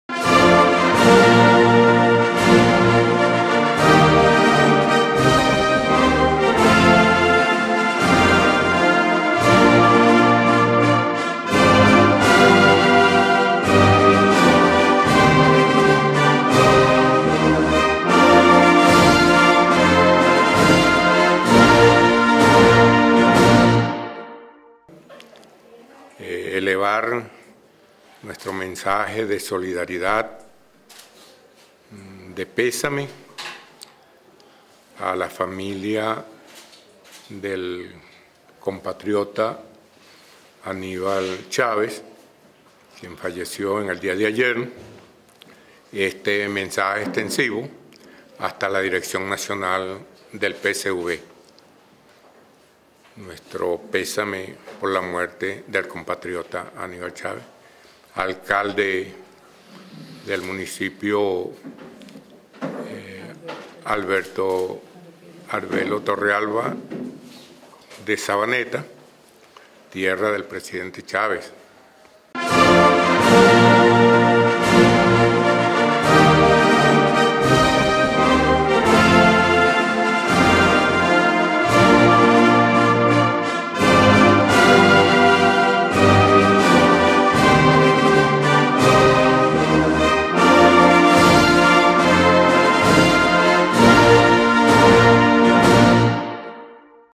PCV expresó su solidaridad por el fallecimiento de Aníbal Chávez Frías
Caracas, 18 jul. 2016, Tribuna Popular TP.- El Buró Político del Partido Comunista de Venezuela (PCV), expresó hoy su solidaridad con la familia del alcalde Aníbal Chávez Frías, recientemente fallecido y hizo extensiva esta solidaridad a la militancia del Partido Socialista Unido de Venezuela (PSUV), de la cual era su militante.